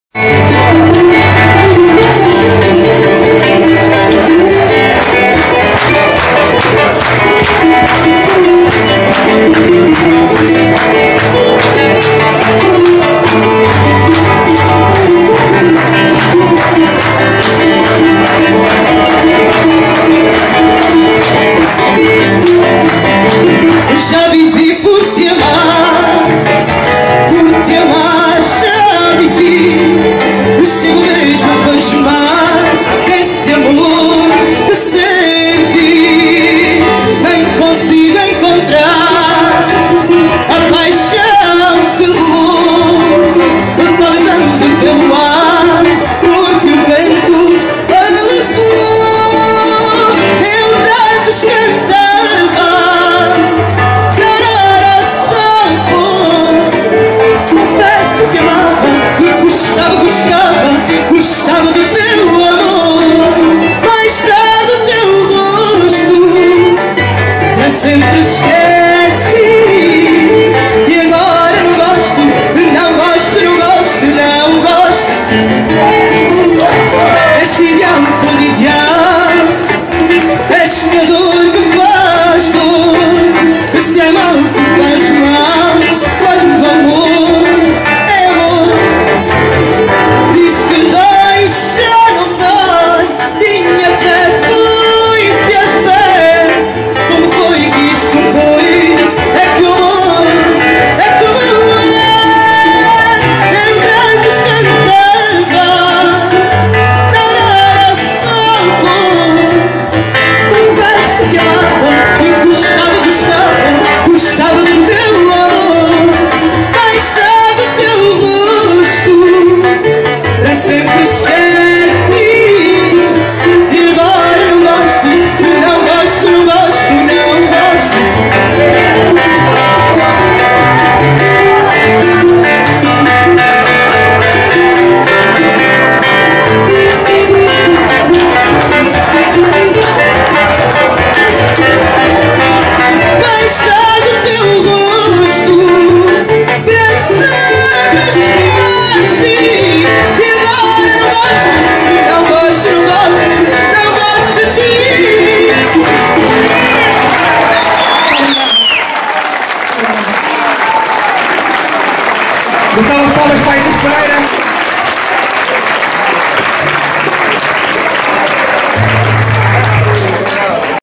FADO in Lisbon - Fadista Inês Pereira - Rua Dos Remedios Fado Festival
Fadista Fantástica!  Inês Pereira -
Rua Dos Remedios Fado Festival. 29 de julho de 2017.

Fado is a music genre that can be traced to the 1820s in Lisbon, Portugal, but probably has much earlier origins. The word "fado" comes from the Latin word "fatum" from which the English word "fate" also originates. Fado is often characterized by mournful tunes and lyrics, often about the sea or the life of sailors, dock workers, port traders and other working-class people in general, and infused with a sentiment of resignation, fatefulness and melancholia. This is loosely captured by the Portuguese word "saudade," or "longing", symbolizing a feeling of loss (a permanent, irreparable loss and its consequent lifelong damage). On 27 November 2011, fado was added to the UNESCO Intangible Cultural Heritage Lists.